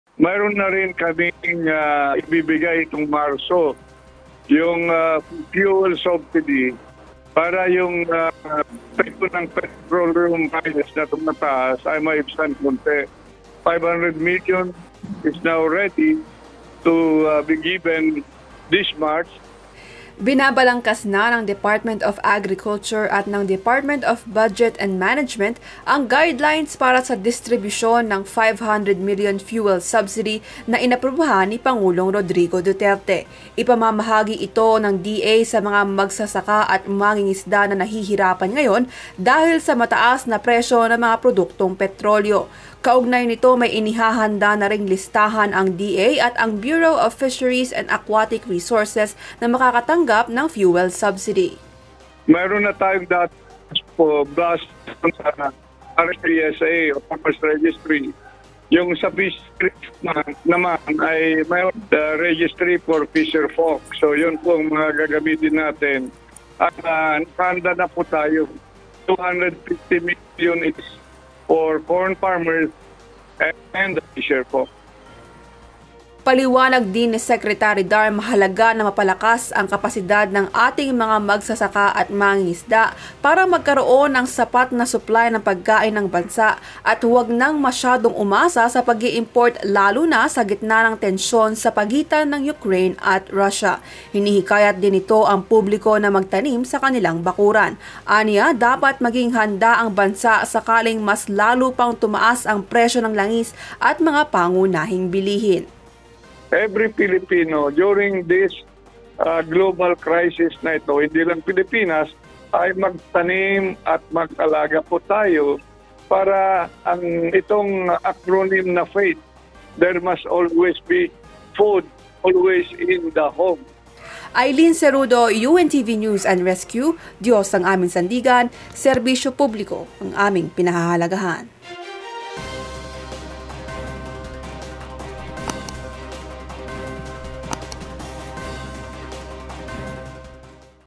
Fuel subsidy sa mga magsasaka at mangingisda ipamamahagi ngayong Marso
Ipamamahagi na ngayong Marso ng Department of Agriculture ang P500 milyong halaga na fuel subsidy na inilaan para sa mga magsasaka at mangingisda na apektado rin ng patuloy na pagtaas ng presyo ng langis.